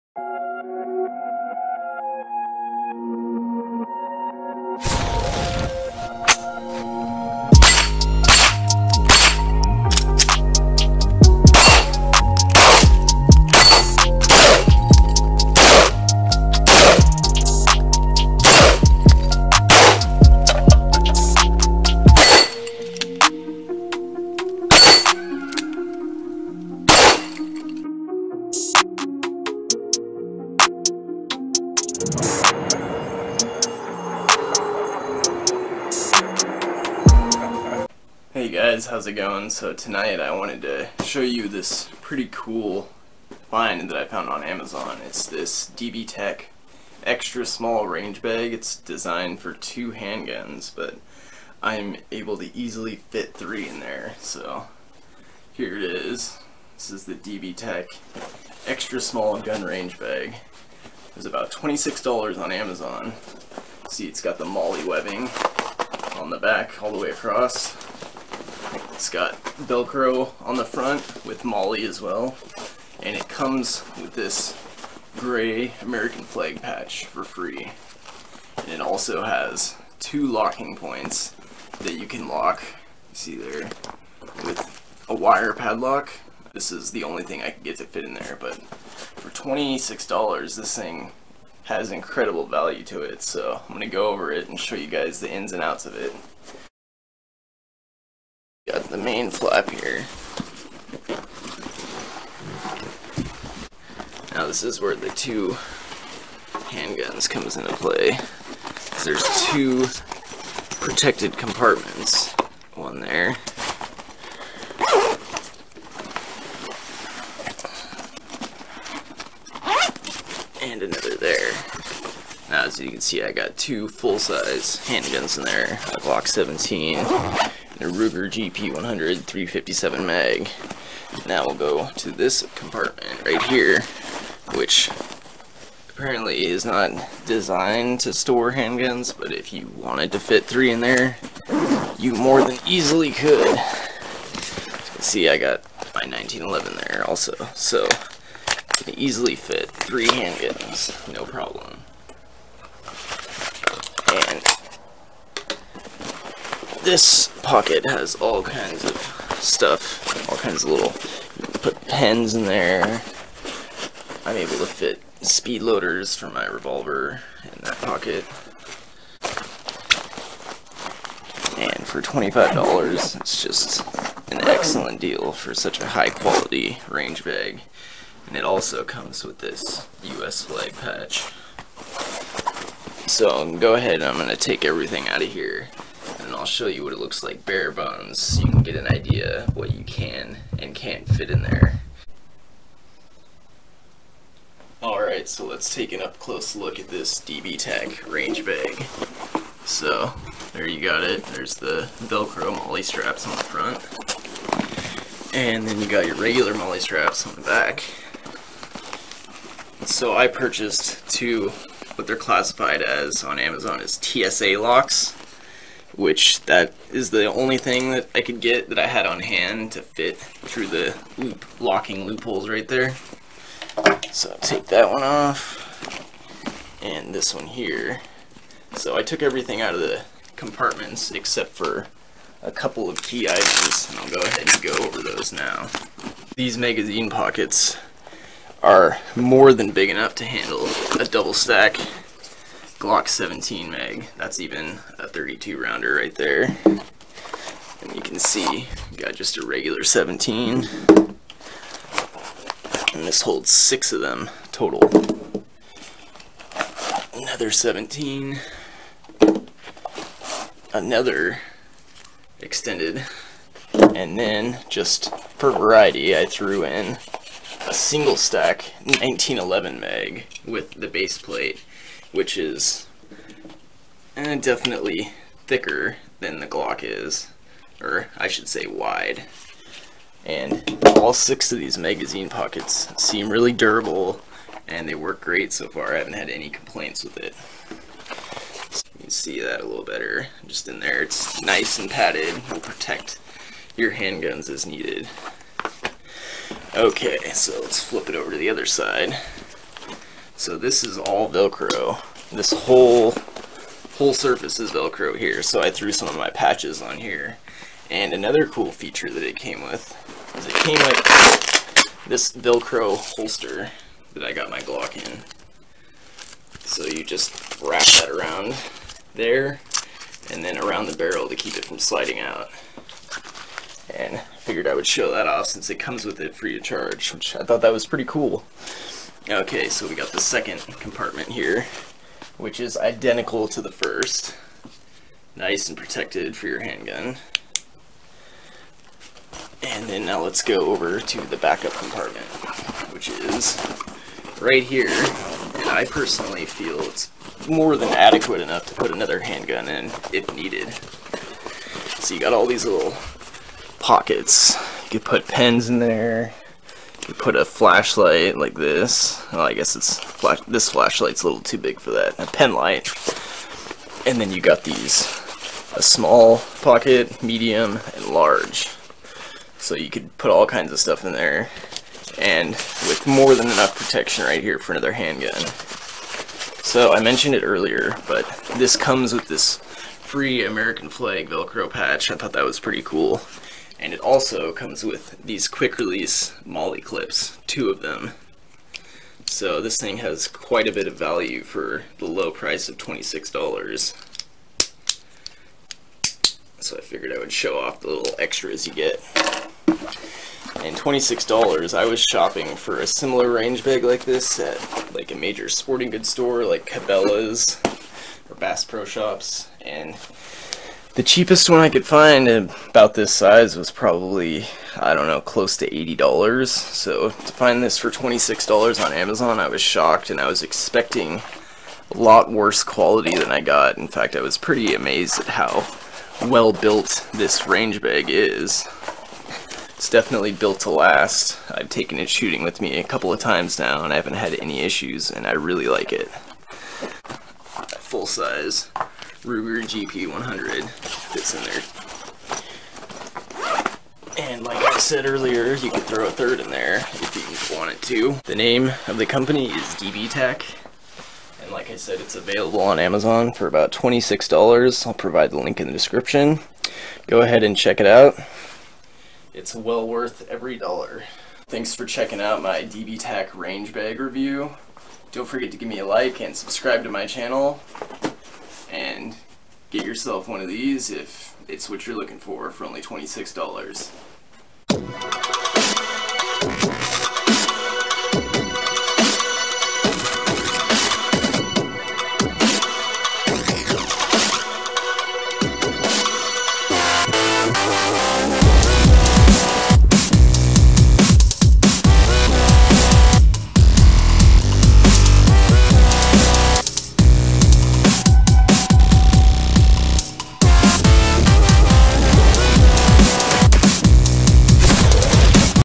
dbtac small handgun range bag review
I review the DBTAC small handgun range bag available on amazon for only $26.99. This DBTAC range bag is classified as extra small and is designed to safely carry and store 2 handguns and 6 magazines although the side compartment can act as a third pocket allowing you to easily store a 3rd handgun inside. Below is the link to this item on Amazon if your interested in purchasing it for yourself.
note: this is the 2nd time this video was uploaded. The original video had a audio error so i deleted it so i could fix my editing mistake and re-upload it.
and here is the link to the TSA combination wire locks for $5.97